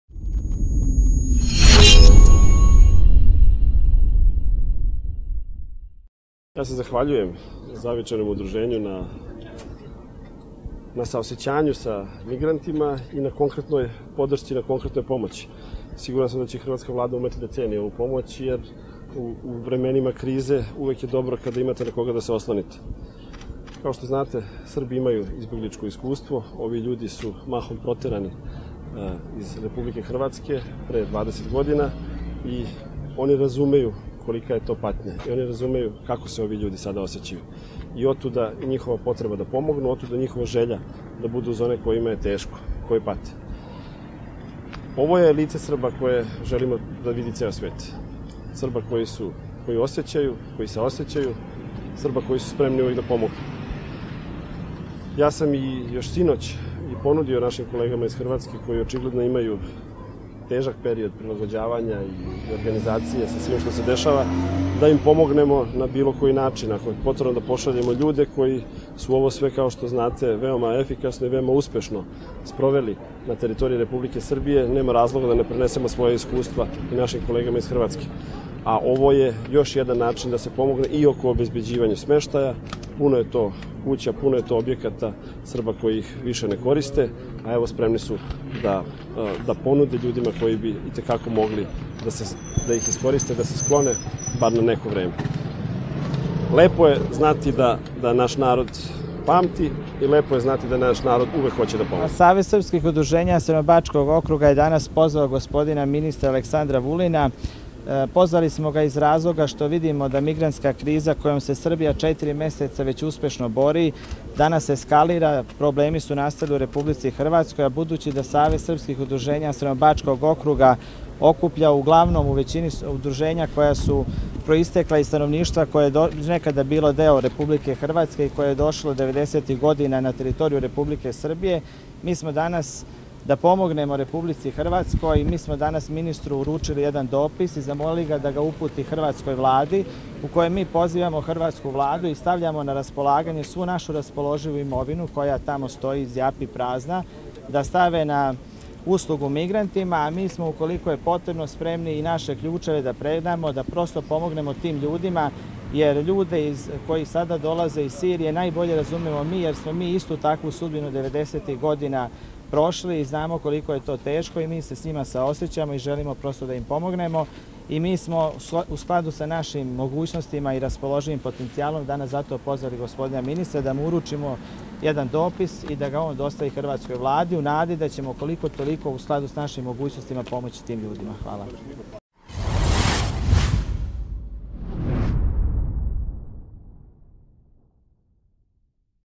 Izbegli iz Hrvatske nude kuće izbeglicama sa Bliskog istoka
SUBOTICA - Izbeglice iz Hrvatske koje žive u Subotici ponudili su danas svoje kuće u Hrvatskoj izbeglicama koje su se bežeći od ratova na Bliskom istoku našli i u toj zemlji.

Predsednik Saveza srpskih udruženja Severnobačkog okruga Saša Gravorac predao je ministru za rad, zapošljavanje, boračka i socijalna pitanja Aleksandru Vulinu dopis i zamolio ga da zvanično bude upućen Vladi Hrvatske, u kojem izbegli iz Hrvatske nude svoje kuće i druge objekte izbeglicama sa Bliskog istoka.